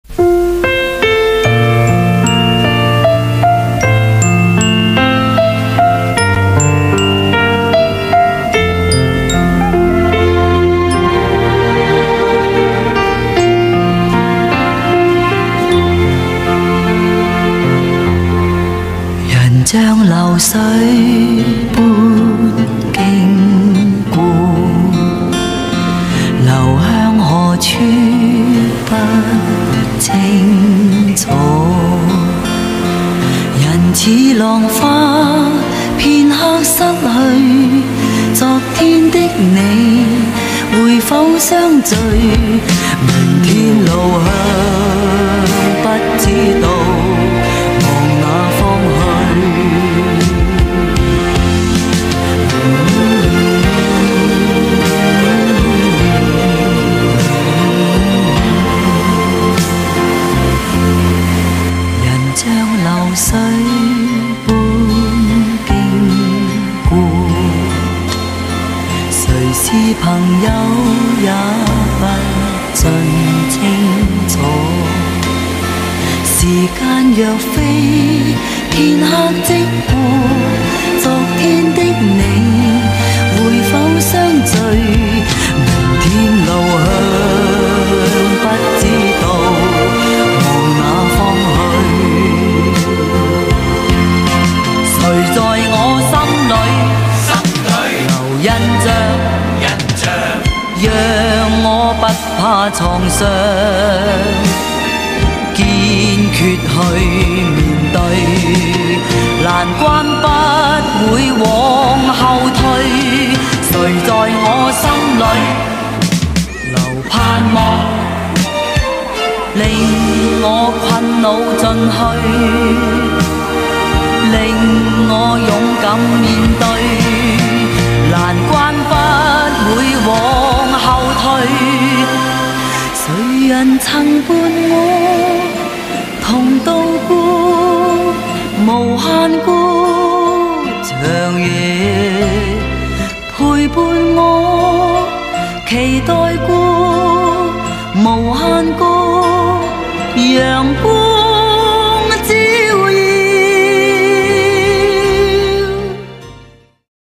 人似浪花 8-10-2022
這兩星期 大清水 都是吹很強風 很大浪 泳季應該快結束 近期開始搜集返家鄉資料 預計十一月中左右出發去東京高尾山看紅葉 明治神宮外苑看梧桐黃葉 應已打了兩針 用長者醫療券做核酸檢查 機票加酒店(二級)約6000元 加其他費用 估約共花一萬元?